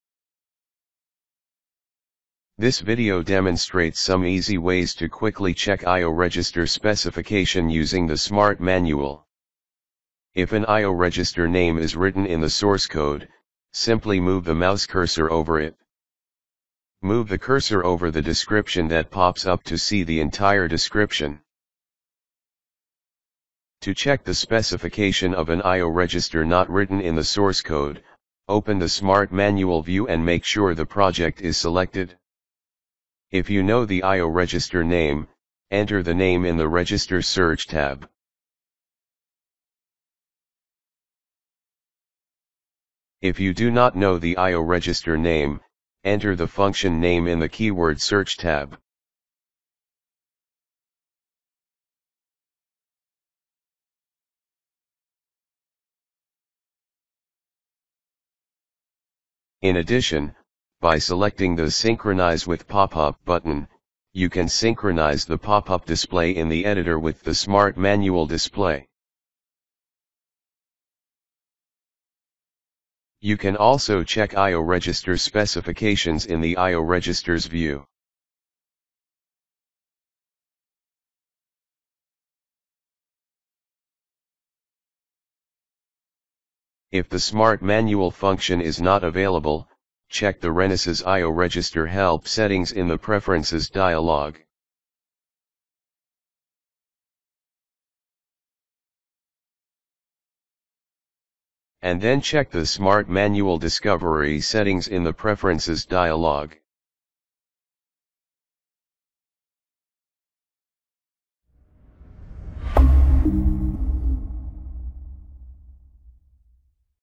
e² studio Tips - Easy Way to Check IO Register Specification (Using Smart Manual)
During coding or debugging, you may want to check IO register specifications. In such cases, you can easily check it on e² studio without having to search in the hardware manual. This video shows some easy ways to quickly check IO register specifications using the Smart Manual.